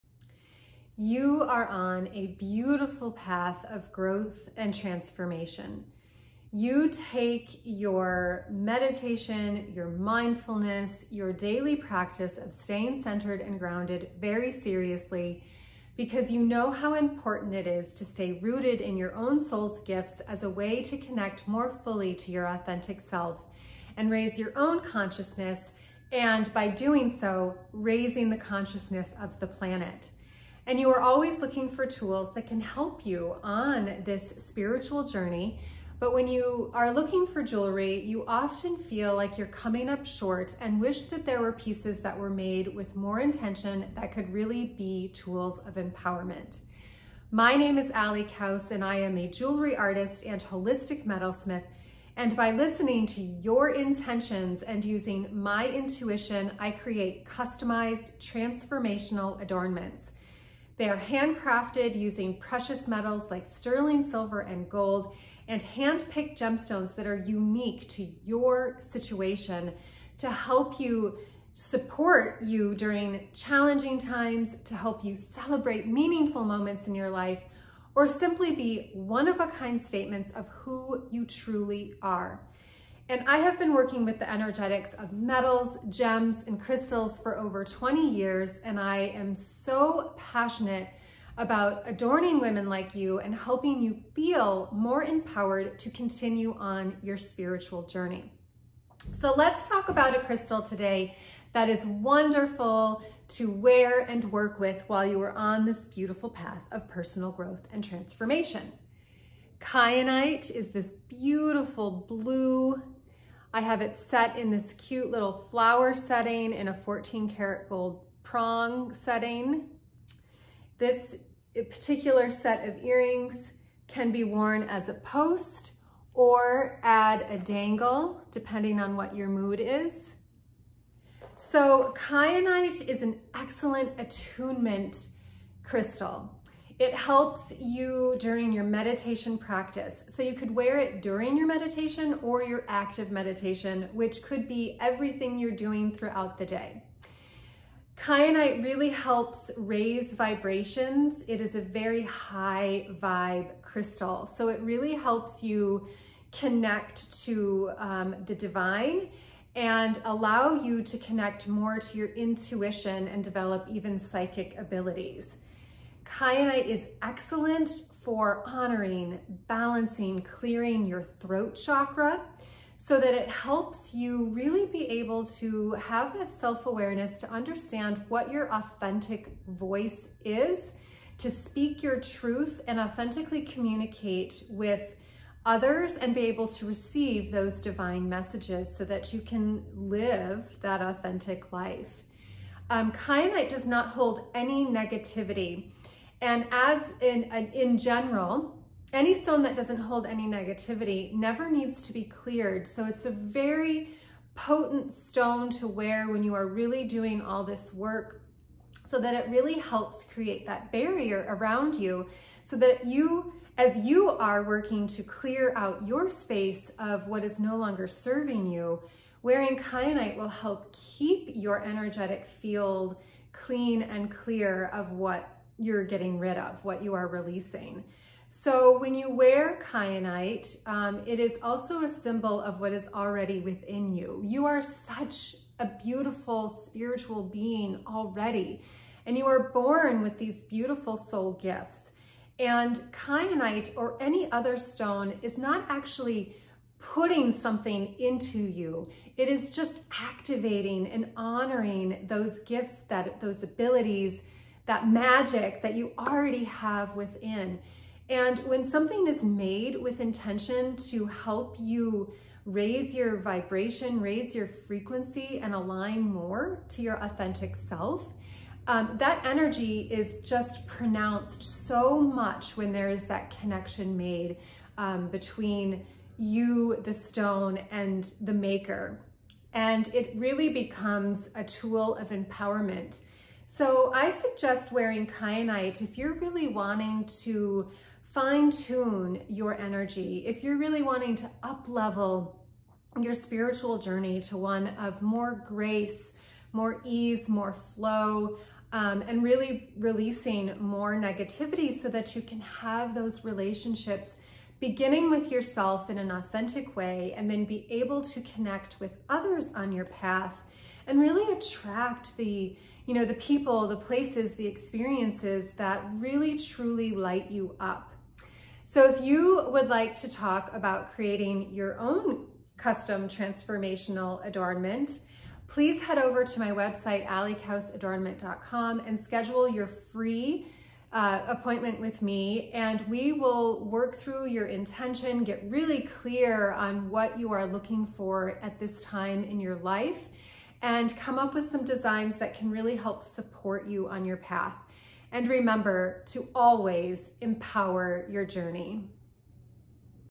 Soothe Your Mind, Body, & Soul with Kyanite
Who doesn't need a calming tonic, even if you have a spiritual practice, it is still so important to know what other tools can help you on your path.

Do you have any Kyanite jewelry? If you did, it would help you raise your vibration, increase your intuition, help you speak your truth, and keep you calm, cool, and collected.

Wearing a hand crafted piece of jewelry  with this gorgeous healing crystal will activate your ability to vibrate higher and match the frequency of all those beautiful things you want to manifest.

Learn more by watching the video!

Each Transformational Adornment is a one of a kind statement piece created with the intention to remind you of your soul's gifts. If you feel like these amazing pair of earrings are for you, they probably are!